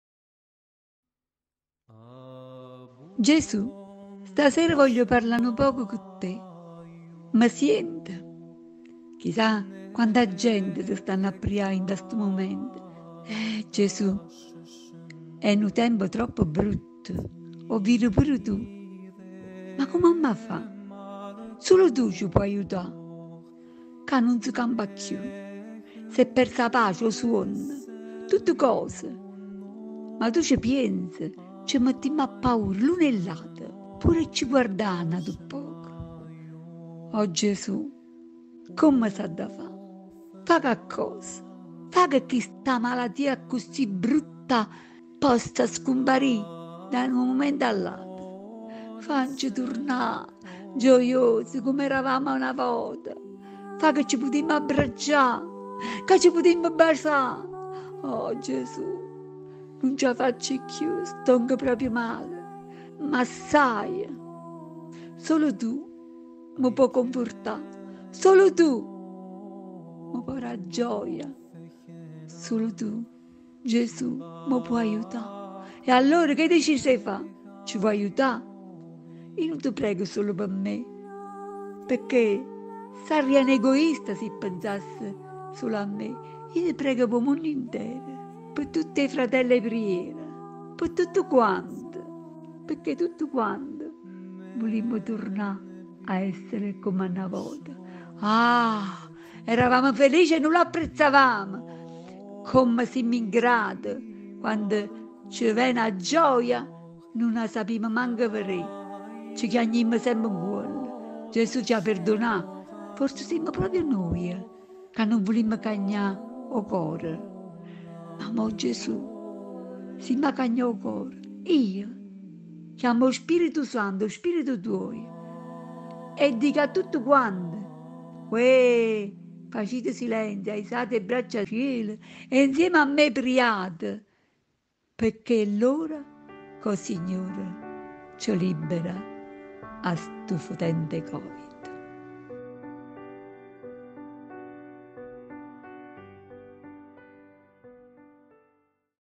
Gesù, aiutece Tu! - preghiera in lingua Napoletana
Preghiera in dialetto napoletano… quasi una poesia! @PreghiamoInsieme  Napoli in the World